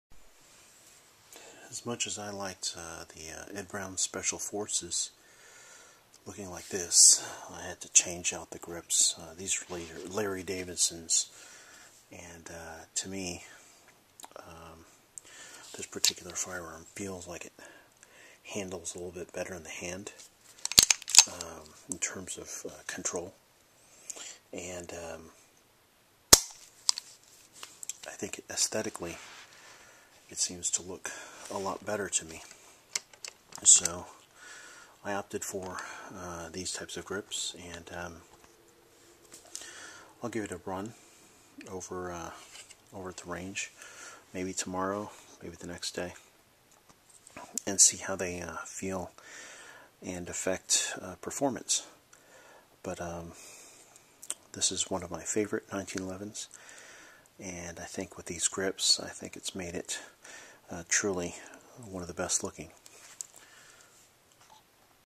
Ed Brown Special Forces, Commander, with Larry Davidson Radial Bobtail grips in G10: 1911 45ACP
The regular walnut grips were OK.  However, the G10 Larry Davidson grips are AWESOME.  I guess I love G10 from my Balisong experiences.  These are very comfortable and impart great control.